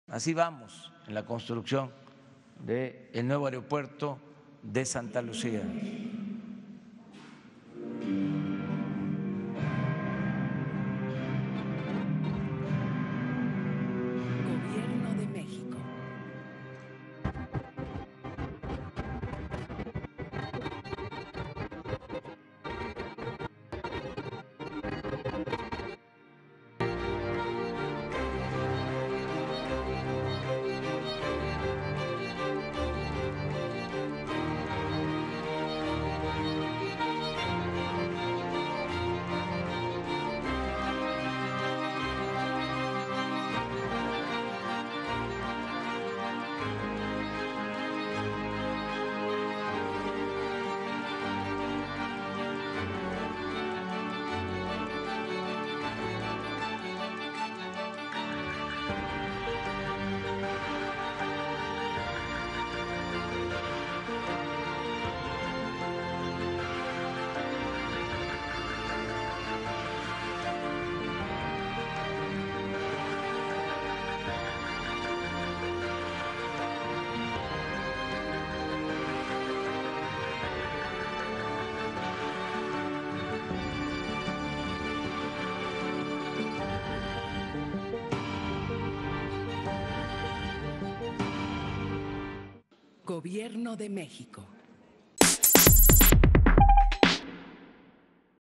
Presentan nuevos avances en la construcción del aeropuerto de Santa Lucia
En conferencia de prensa, el mandatario Andrés Manuel López Obrador presentó nuevos avances en la construcción del aeropuerto de Santa Lucia.